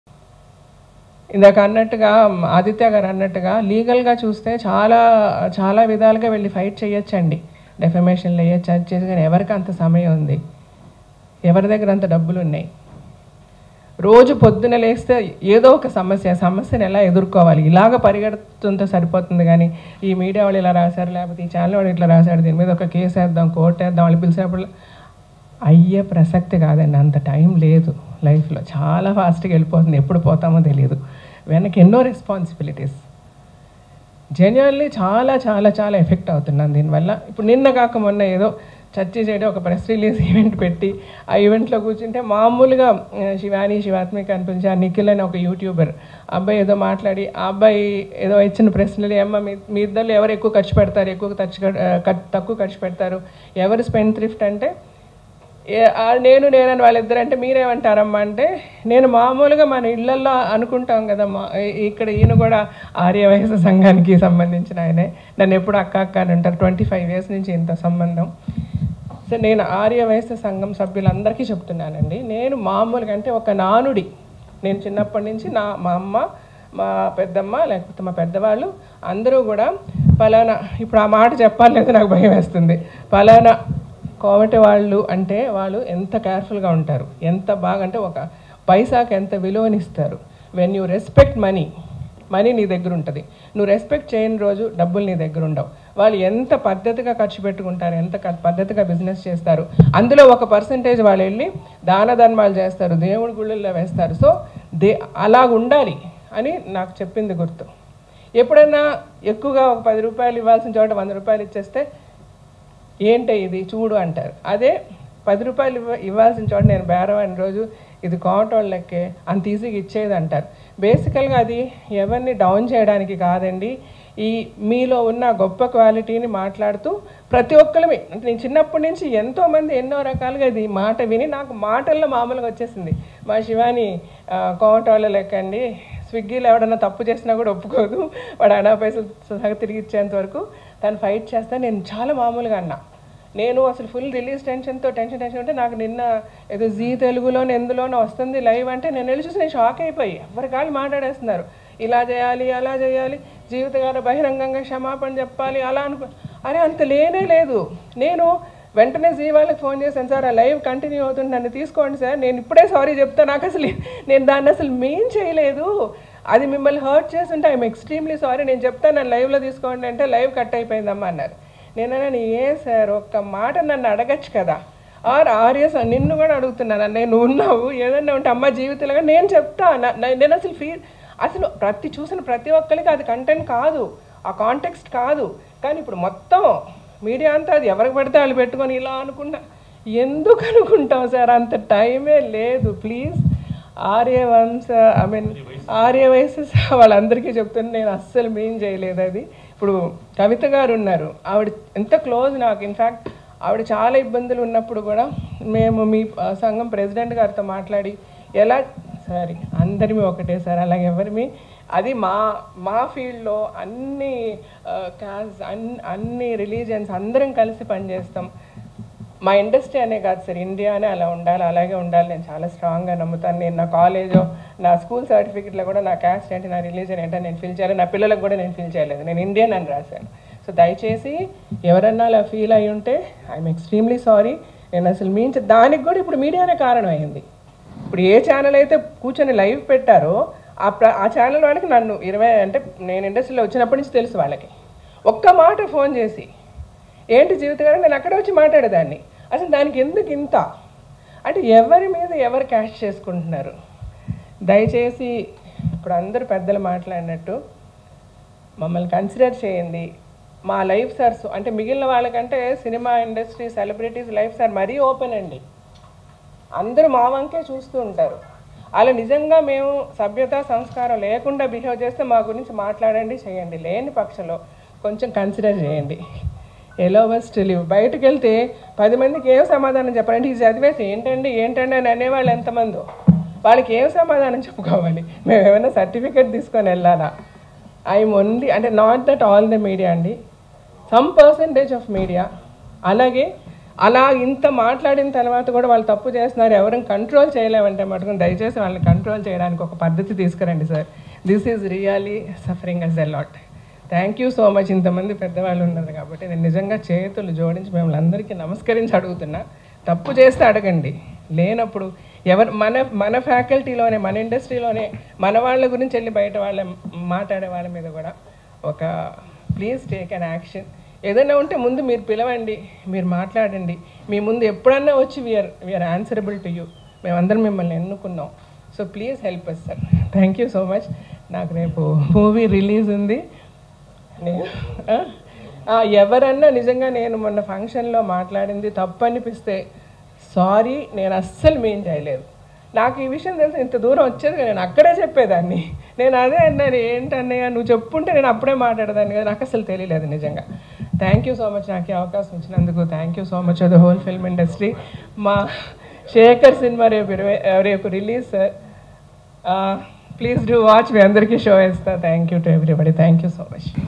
Jeevitha Rajasekhar Says Sorry To Arya Vysya Sangam | Manastars
Watch JeevithaRajasekhar Says Sorry To AryaVysyaSangam